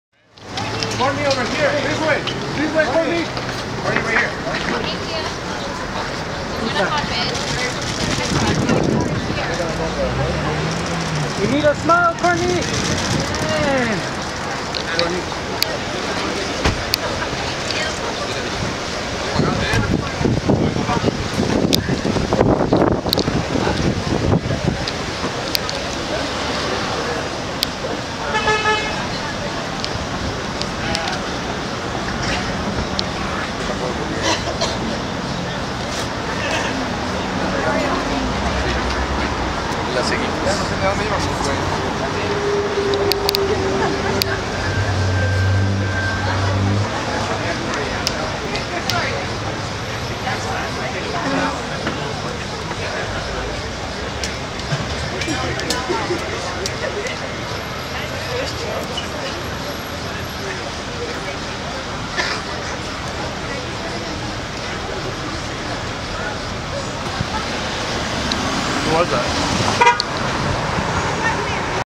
Kourtney Kardashian Dons Sunglasses and Hoodie While Fleeing a Paparazzi Mob in her Mercedes
While sister Kim is busy posting naked selfies on the internet, big sister Kourtney Kardashian does her best to cover up while out in Beverly Hills, Ca

The eldest "Keeping Up With The Kardashians" sister was out having lunch at family favorite Il Pistaio Restaurant and decided to don her best Unabomber look as she headed back to her car while getting besieged by paparazzi and fans alike.

For a family that enjoys being in the public eye , we guess there are days when they just ain't feeling it.

*DO NOT DOWNLOAD OR RE-UPLOAD WITHOUT PERMISSION*